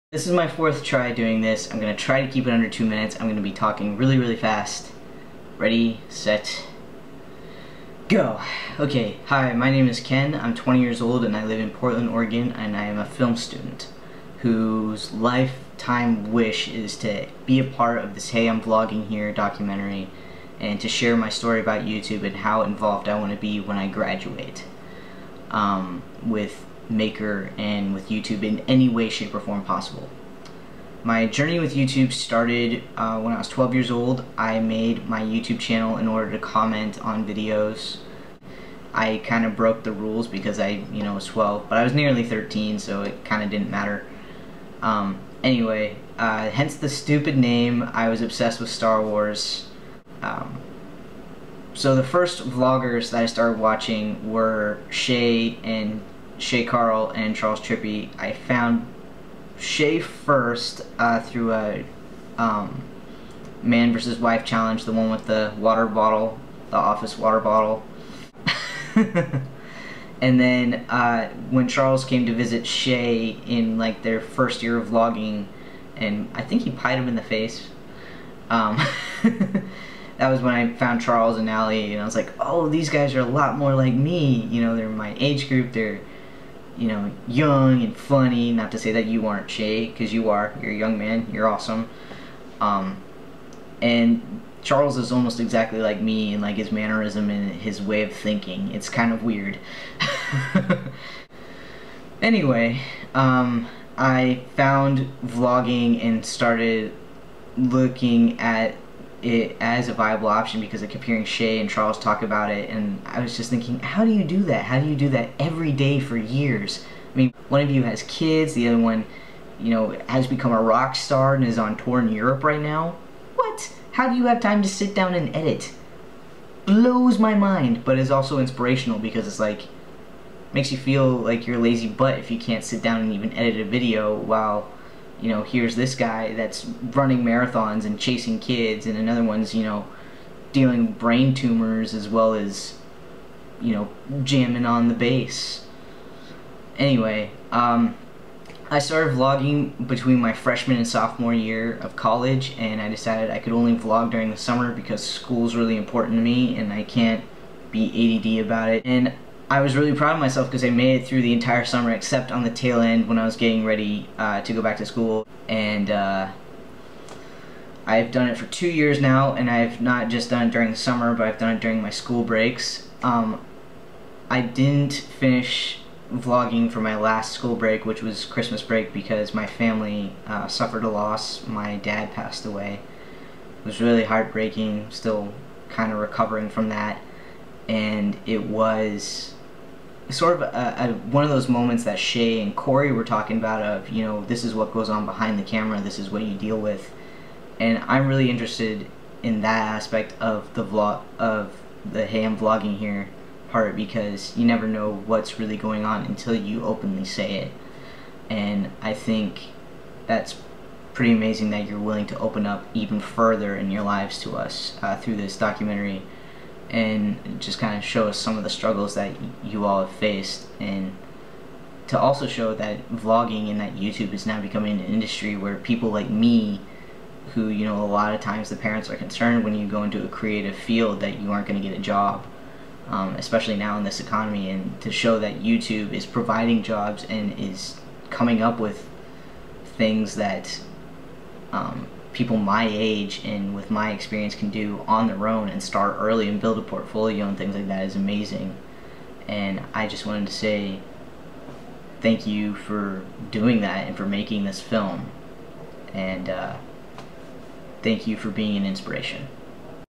HEY I'M VLOGGING HERE RESPONSE! (My Youtube journey can't be explained in 2 minutes)
Hey Shay I really hope you watch this! I know its longer than you want but my journey has been just as long as yours.